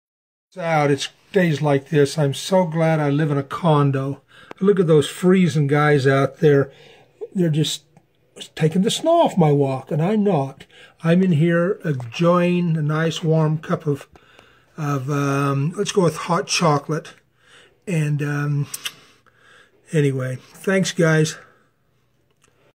Dennis Ward Memories and Videos
Over the last several years of Dennis' life, he often made short videos talking about what was going on in his life.